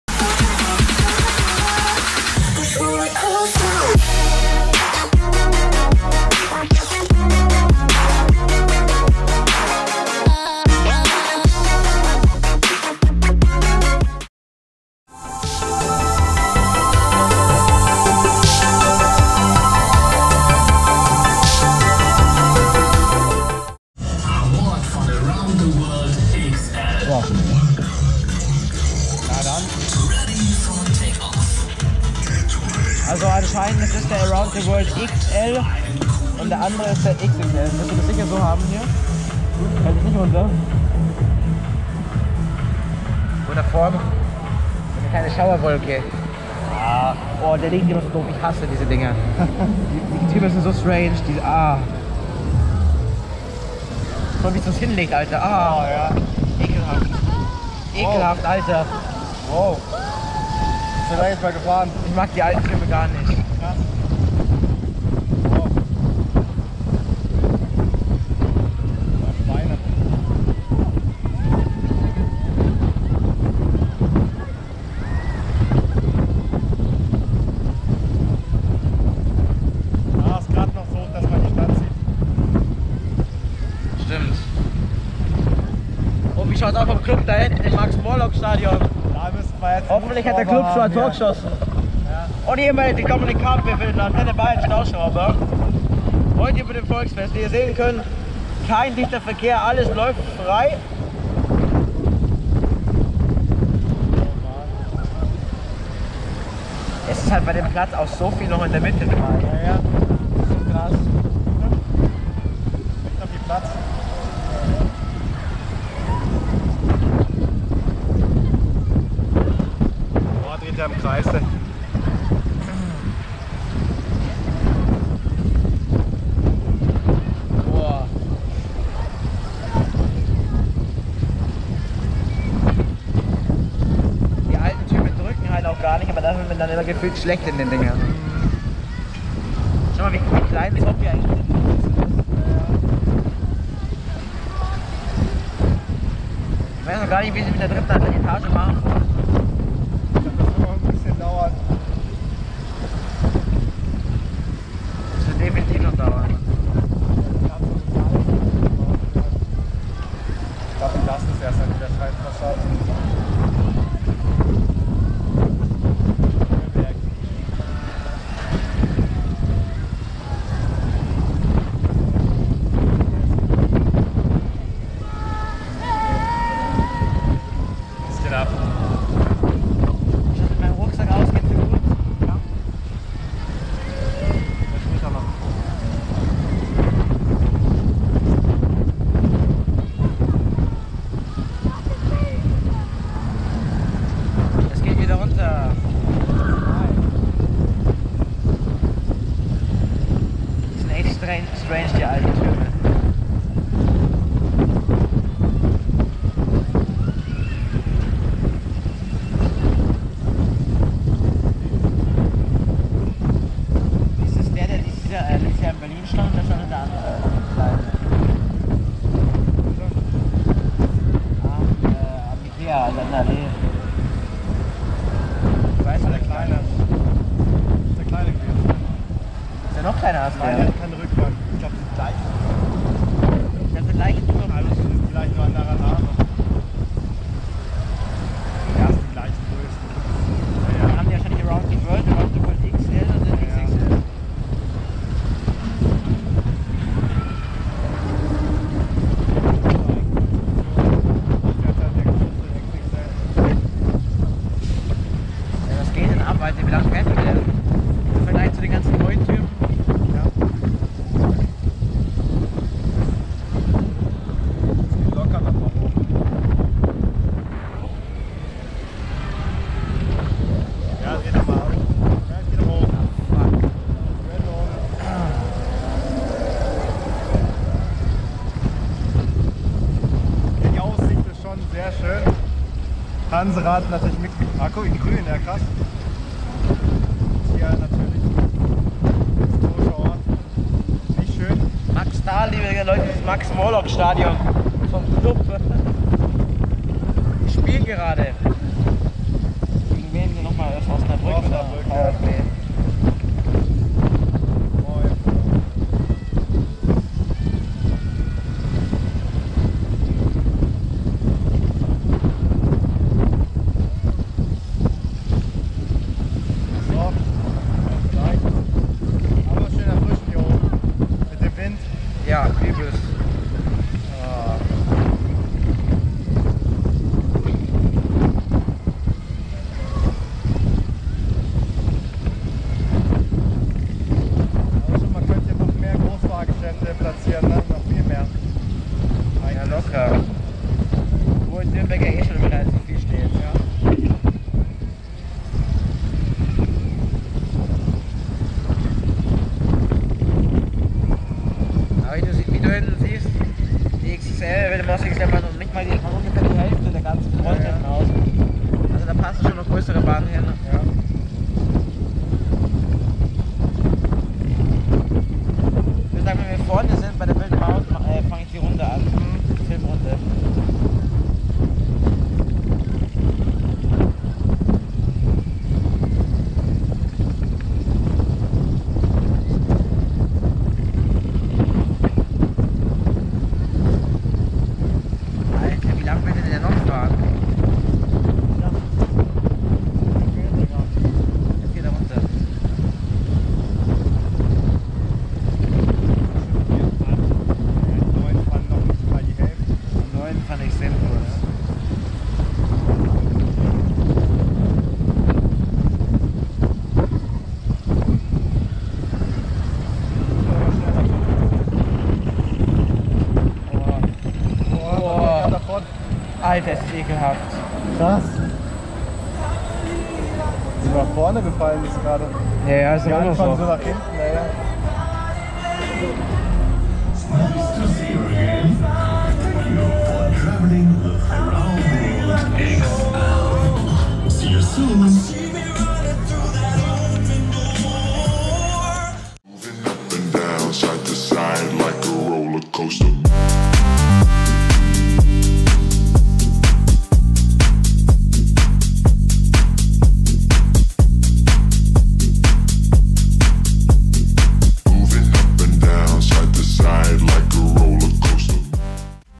Around the World XL - Van der Beek/Dauphin onride | Herbstvolksfest Nürnberg 2019
Der kleine Turm der Familie van der Beck heißt jetzt Around the World XL. Was ich an den Türmen des Typs besonders lustig finde ist, dass sich die Sitze beim Bremsen und Beschleunigen stark nach vorne bzw. hinten lehnen. Im Vergleich zu den 80 Meter Türmen fährt dieser auch langsamer und deutlich länger, was das ganze zwar nicht so intensiv macht, man kann die Fahrt aber besser genießen.

Fahrpreis: 5€
Fahrdauer: ca.7min.
Schausteller: van der Beek-Dauphin VOF (Zaandam/Niederlande)
Typ: Kettenflieger
Maße: 19 x 19 x 60 m
Anschluss: 80 kW
Kapazität: 32 Personen in 16 Gondeln

►Kamera 2: GoPro Hero 7

►Kamera 3: Iphone X

Ab gehts in die schönsten Freizeitparks, Schwimmbäder und Anlagen sowie auf die schönsten Volksfeste: TheKirmesCrazy